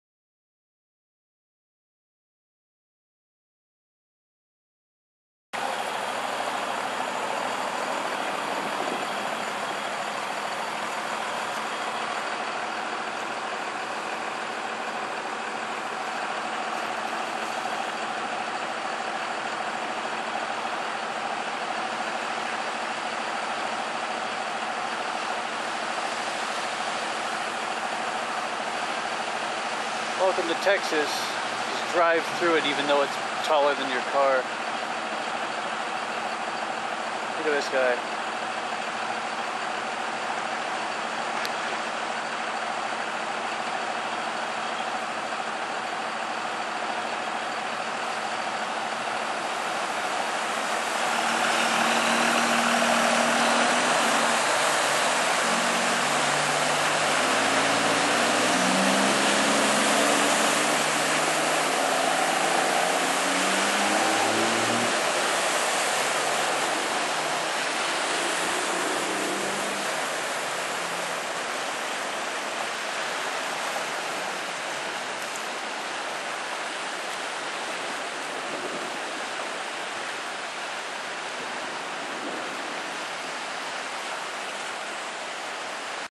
Flash Flooding In Lubbock, Texas. June 1st, 2016.
Driving through an area of extreme rain turned the roads into a temporary river.

Think your buddy would? Share it!